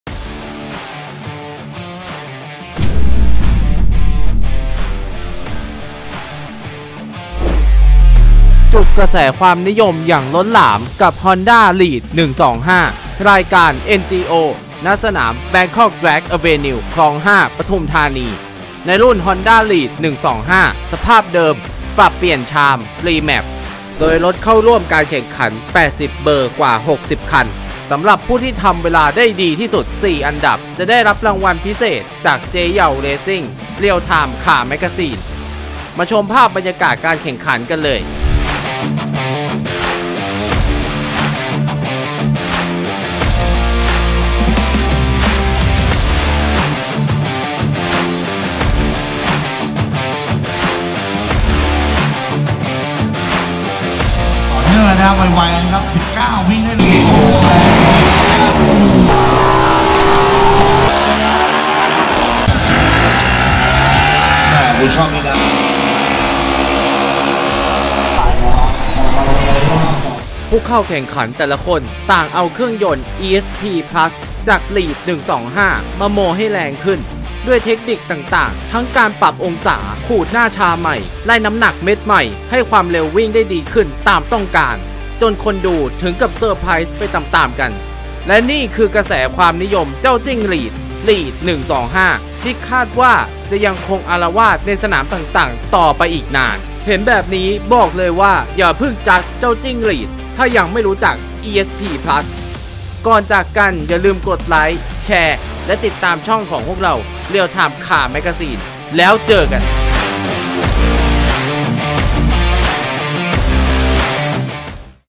ใครจะเชื่อว่า Honda lead 125 จะโมให้แรงได้ถ้าไม่เชื่อมาพิสูจน์กัน ที่สนาม3 NGO คลอง5
ใครจะเชื่อว่า Honda lead 125 จะโมให้แรงได้
ถ้าไม่เชื่อมาพิสูจน์กัน ที่สนาม3 NGO คลอง5
บอกเลยว่า เจ้เยาเรสซิ่งทุ่มสุดตัว!

ชาว Honda lead 125 ที่โมเครื่องแล้ว
มาโชว์รถที่ใต้คอมเมนต์กันหน่อย